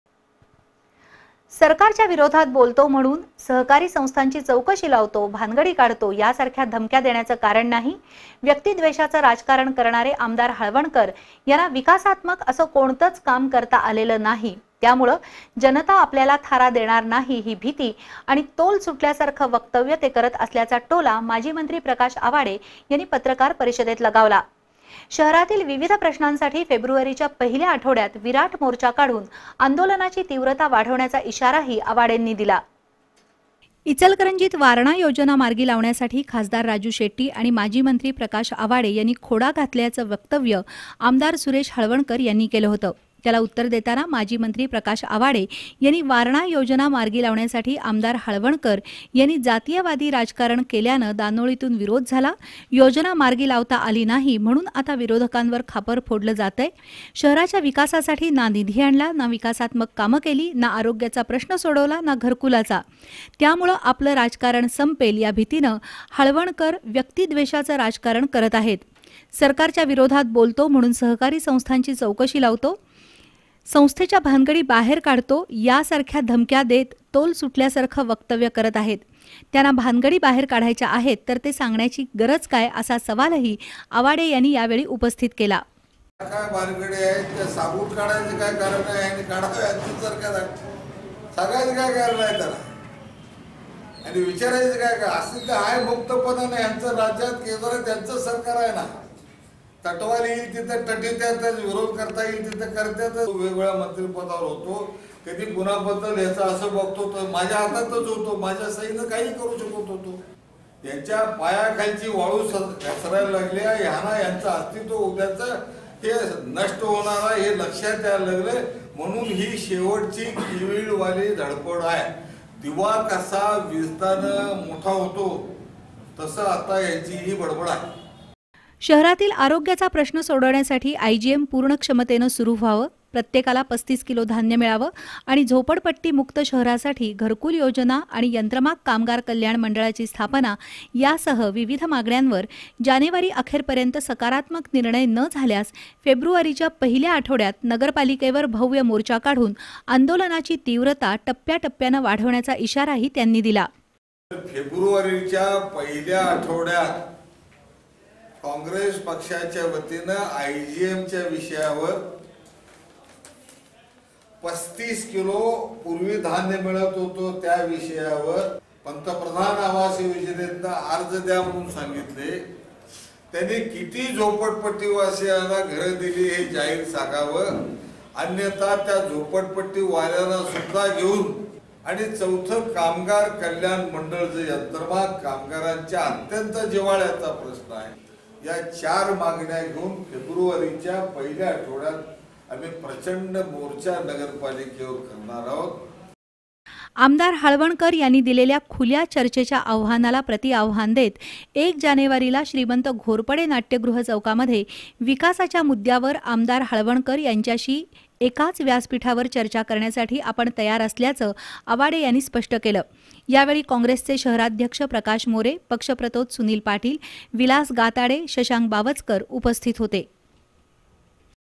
व्यक्तीद्वेषाचं राजकारण करणार्‍या आमदार सुरेश हाळवणकर यांना विकासात्मक चेहरा नाही
व्यक्तीद्वेषाचं राजकारण करणार्‍या आमदार सुरेश हाळवणकर यांना विकासात्मक चेहरा नाही, माजी मंत्री प्रकाश आवाडे यांची टिका
Channel B